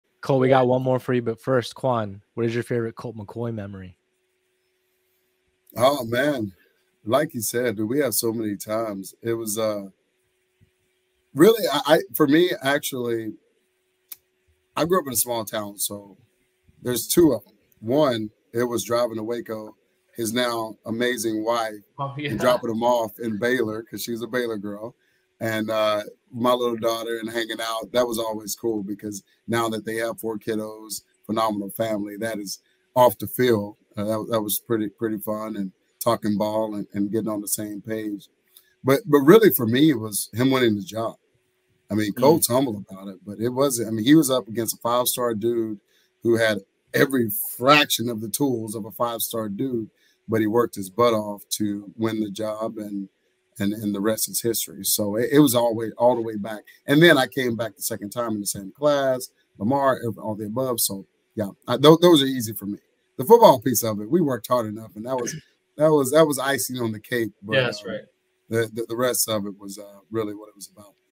Quan' Cosby Shares His Favorite Colt McCoy Memory
Nothing like a QB and WR exchanging war stories, in this one Quan gives his favorite Colt McCoy memory.